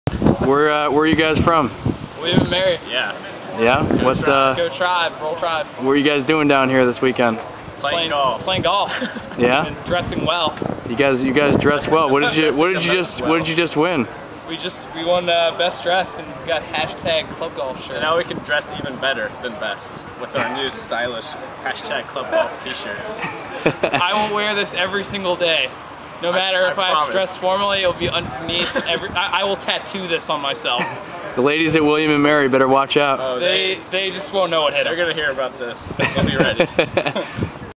William and Mary Golfers on Spring Break
William and Mary student golfers win the fashion contest at the Nextgengolf Spring Break Open and come away with some new clubgolf tee shirts.